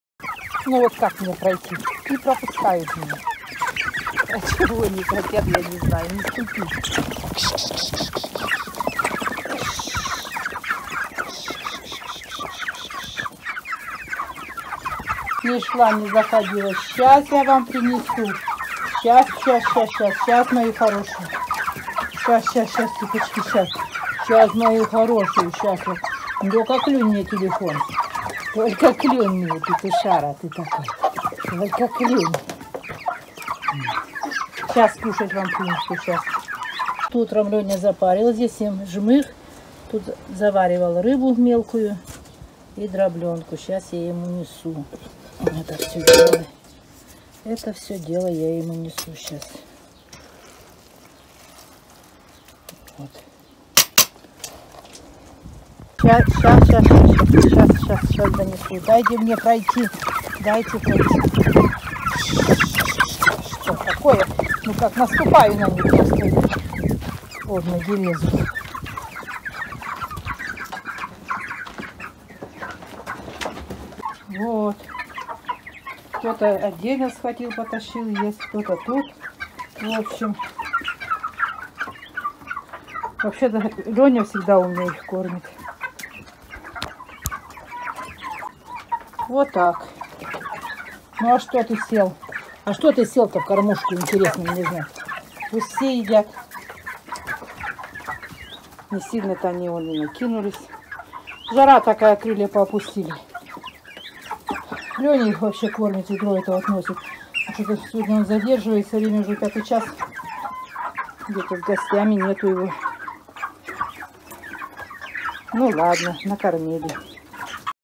Кормушки были пустые
Бройлеры съели корм.Обычно муж приносил им ведро мешанки со жмыхом ,дробленной пшеницей  с добавлением вареной рыбы.Но на этот раз его не было дома и пришлось самой накормить этих обжор!Не дают пройти,  лезут под ноги.